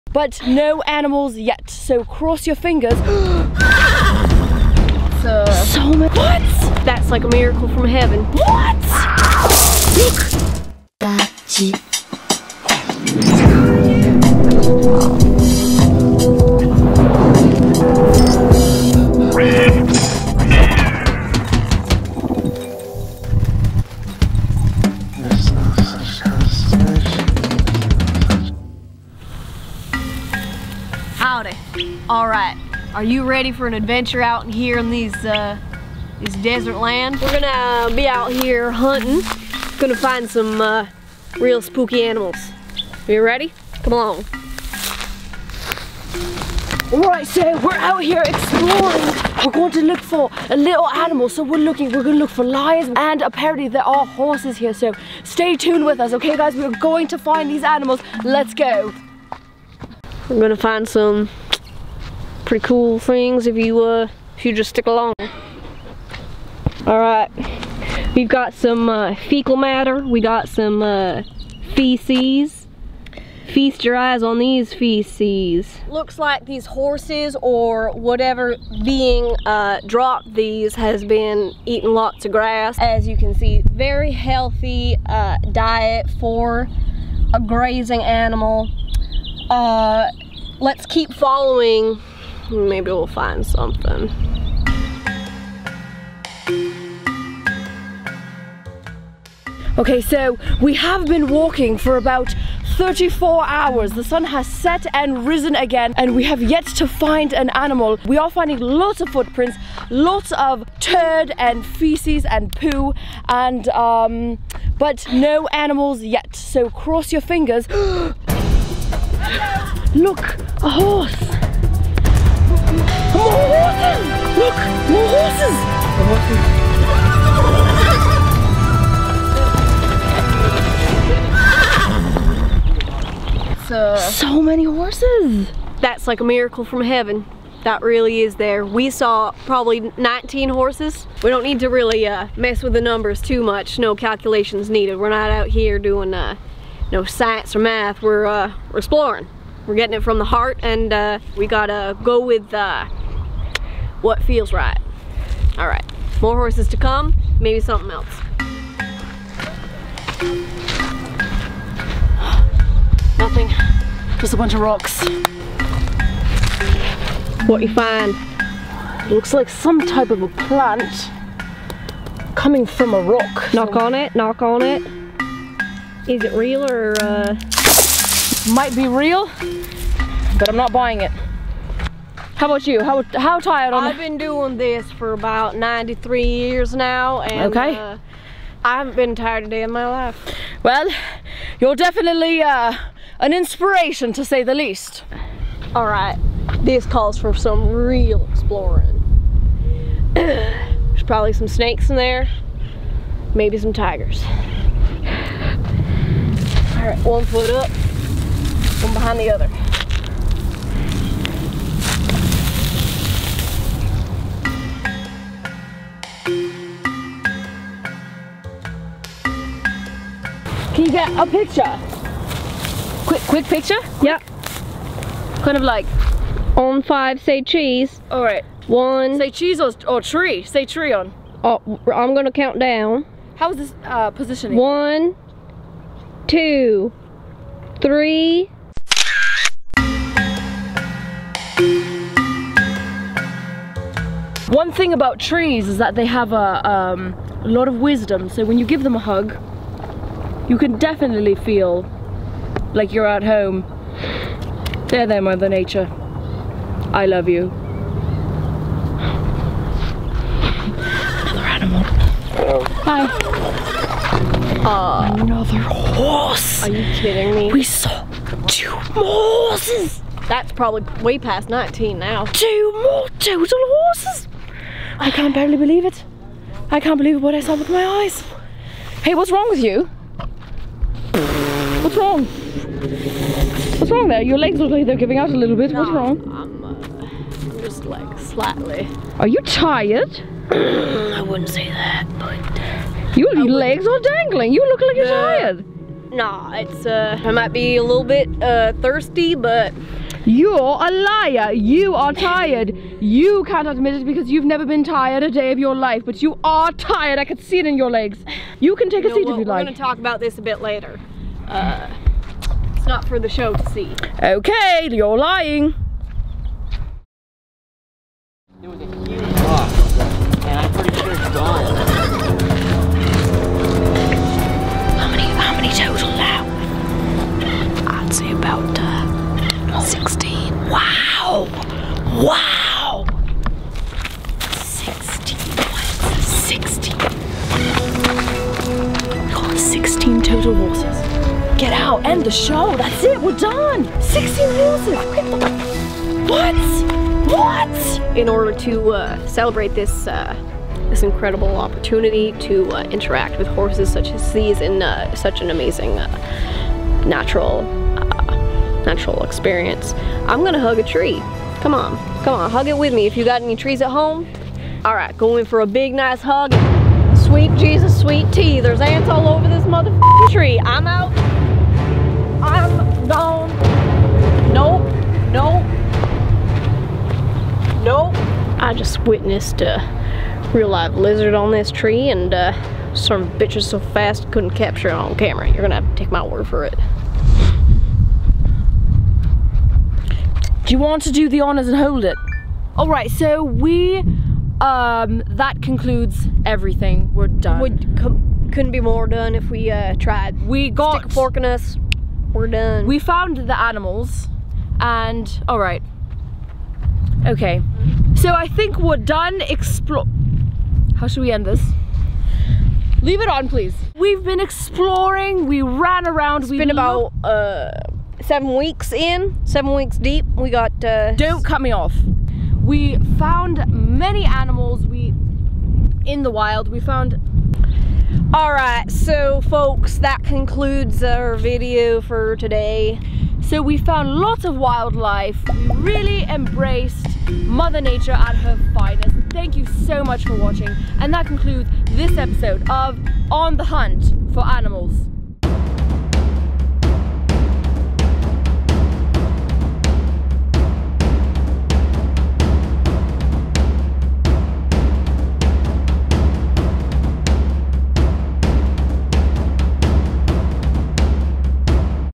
HORSE BACK RIDING?! 🏇 California Desert Adventures
Explore the abandoned wild western desert in the hot California wilderness as these two wild life enthusiasts / zoologists find wild horses and much more. Join the adventure and look and see if you can spot any majestic creatures, plants, and trees. Wow, mother nature is mind blowing!

"Absurd" web series starring Marina Bruno & Jess Reinboldt. EP.1

Join my universe. Become a FILMGEEK

MY VIDEO GEAR ↓
Gorilla Tripod